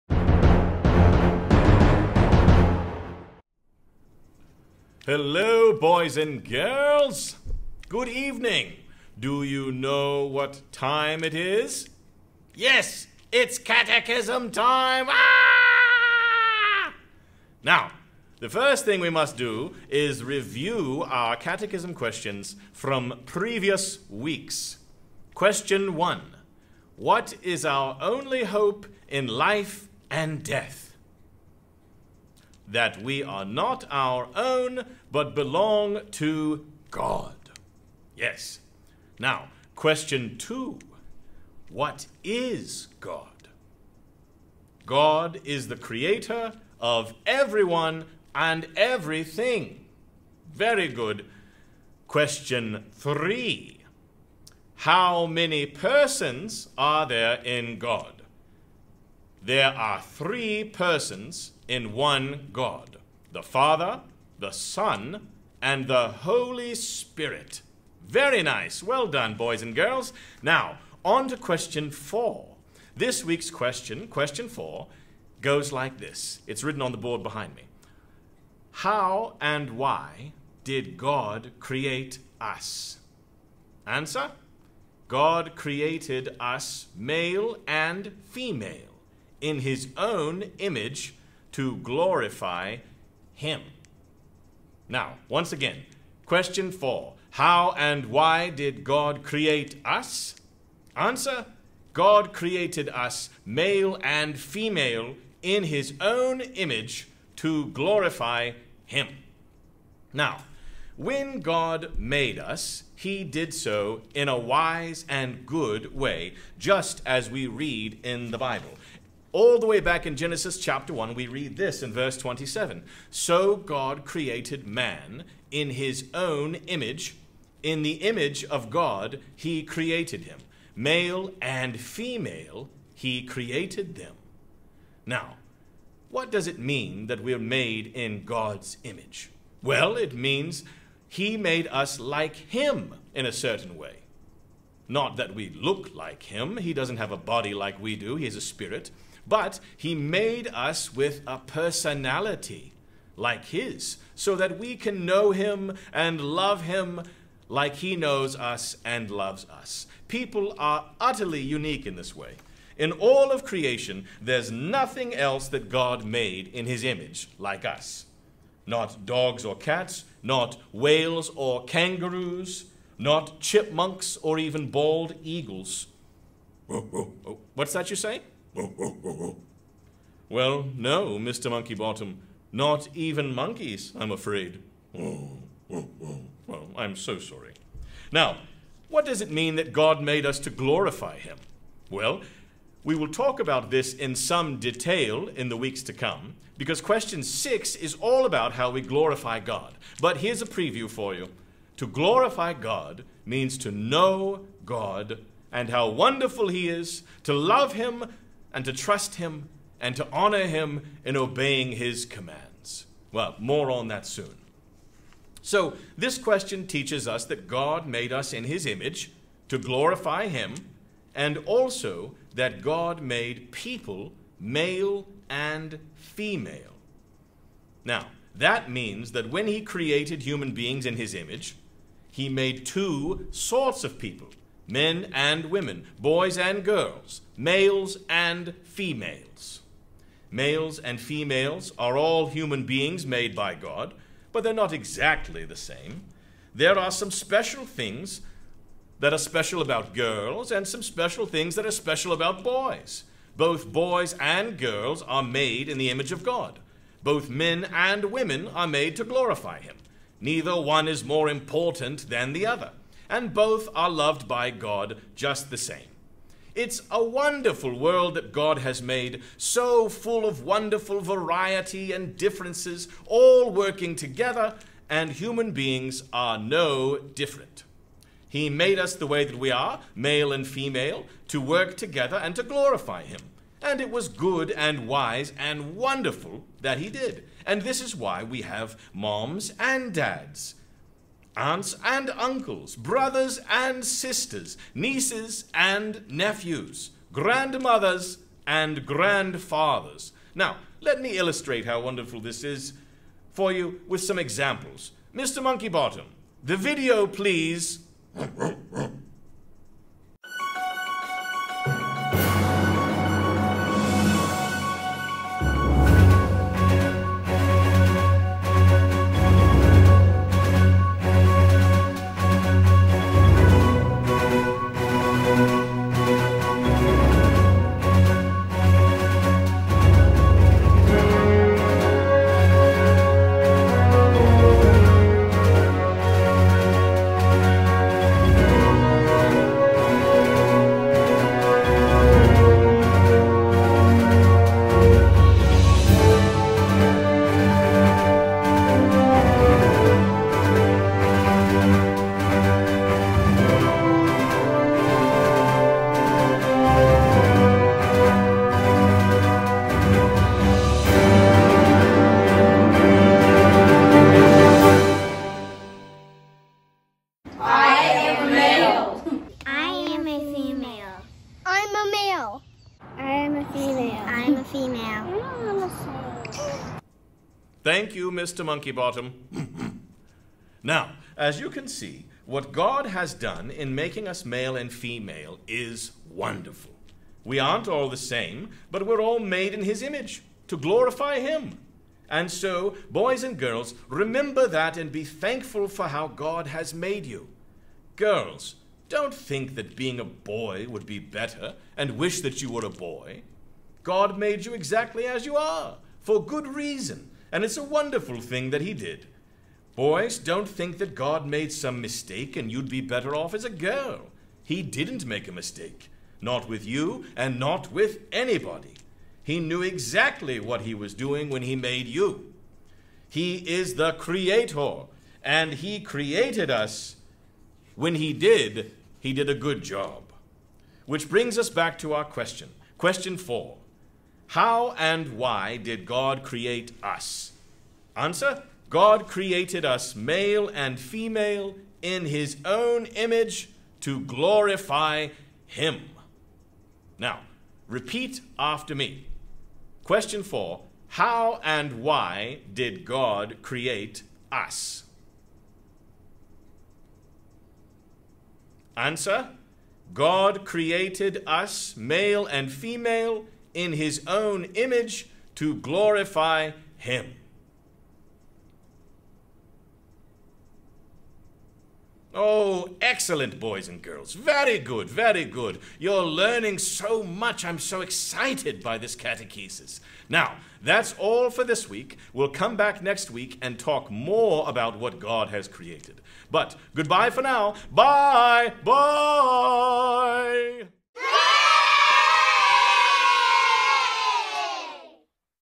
New City Catechism Question 4
Catechism Clarence and Mr. Monkeybottom give an introduction to question four of the New City Catechism.